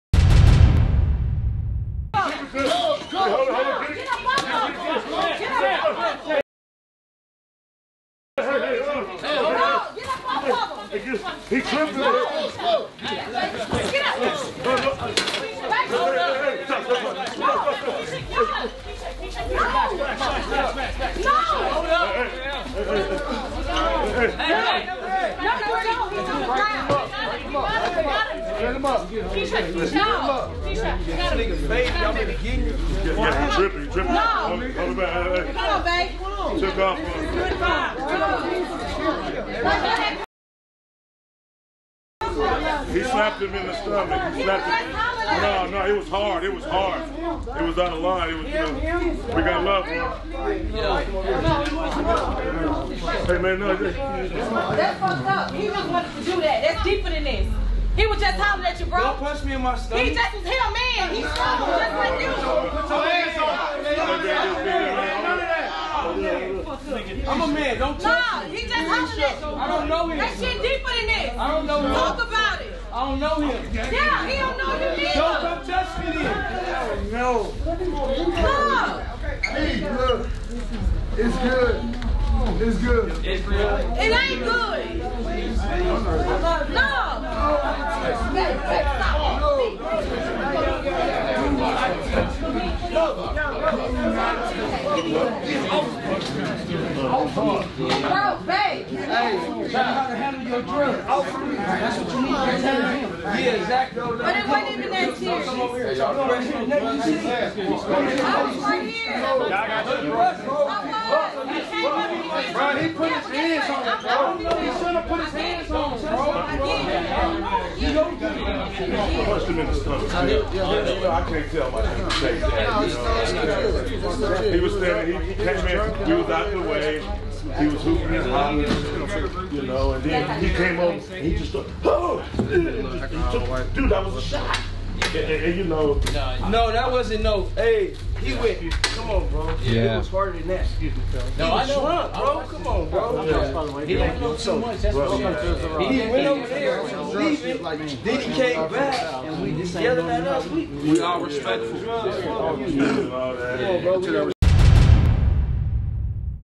Young Buck Gets Into Fight With Afroman's Crew | TMZ
Afroman and Young Buck were recently at odds -- and it got so ugly, hands were thrown ... leading to a full-blown brouhaha breaking out between their respective crews.